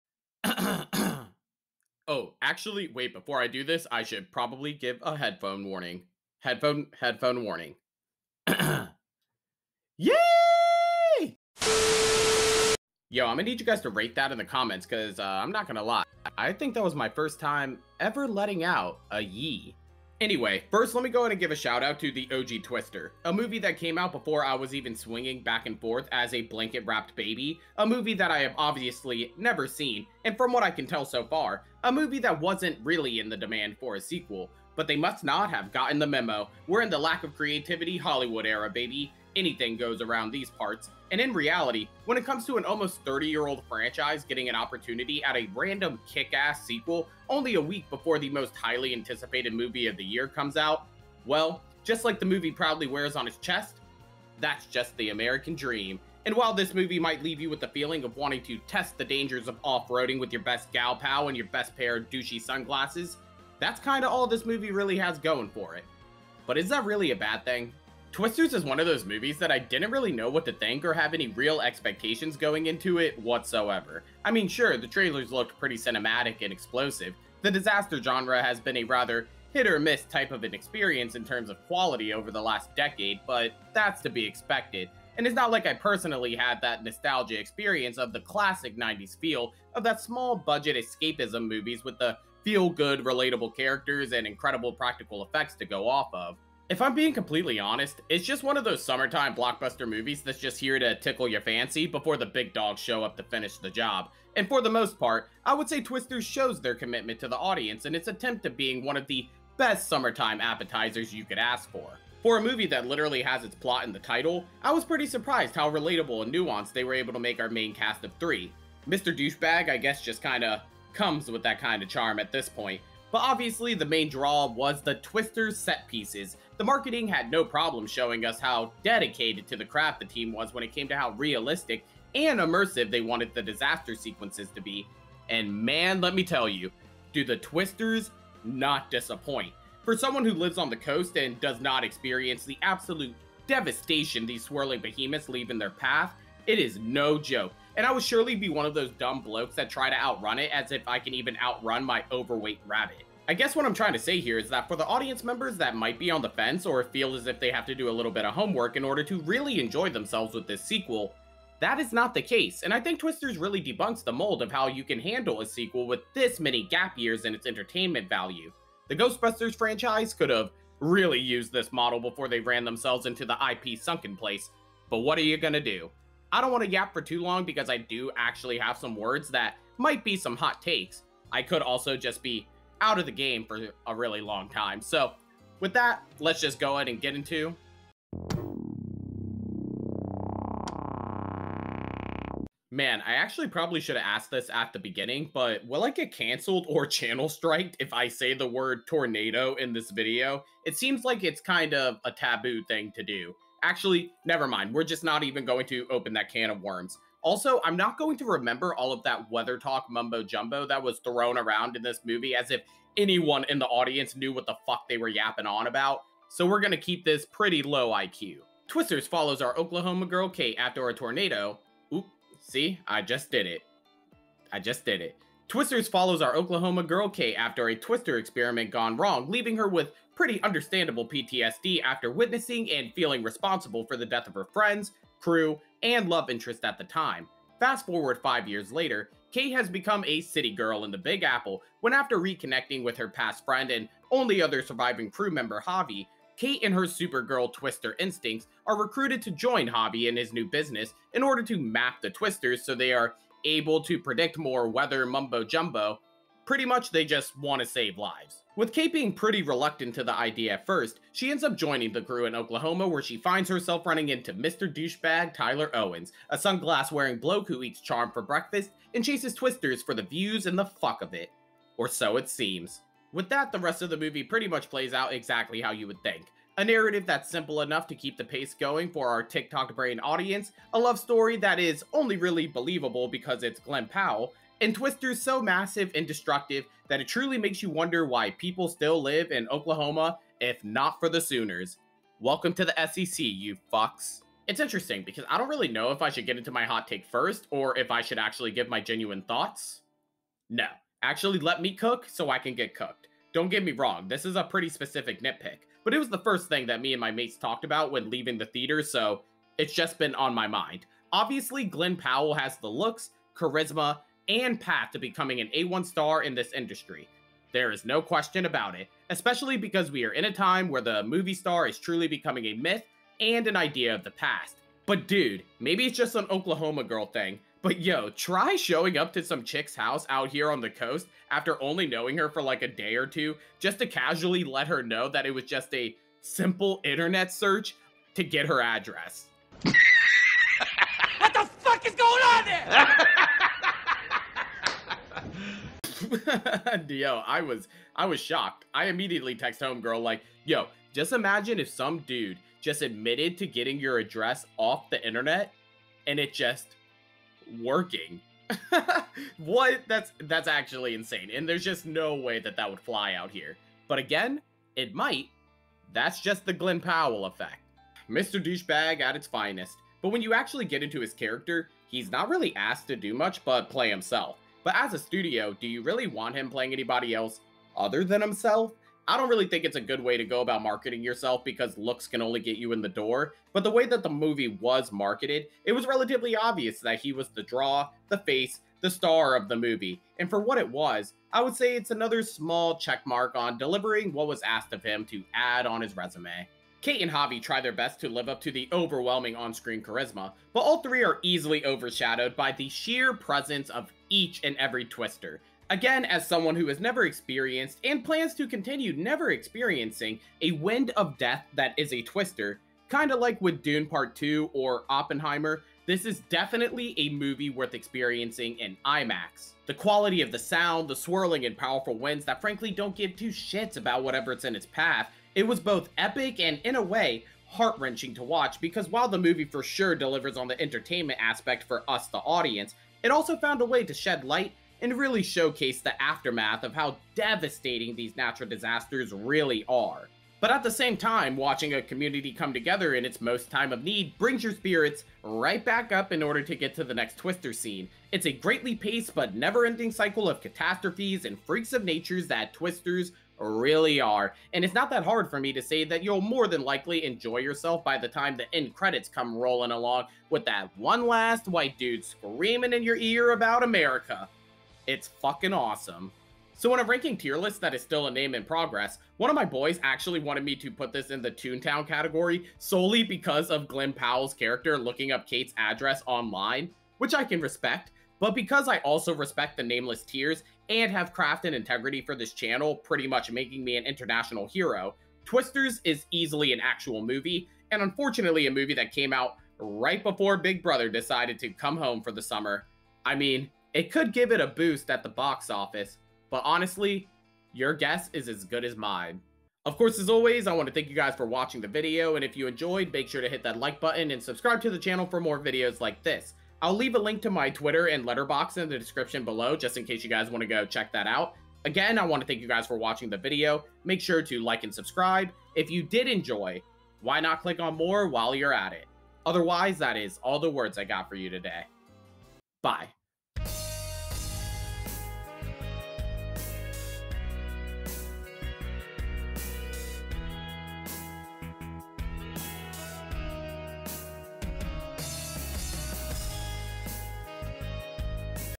Twisters - Is This a Sequel ?
Twisters is an age gap sequel that has so many gap years that I wasn't even a nothing baby. Again for what it was, I do throughly think you'll enjoy yourself by the time the end credits start rolling. MY BAD for hyperfixating on the Glen Powell aspect. it was just... a lil jarring to say the least. Maybe it's an Oklahoma girl thing. But I'll just leave it up to the comments to either cook... or GET COOKED.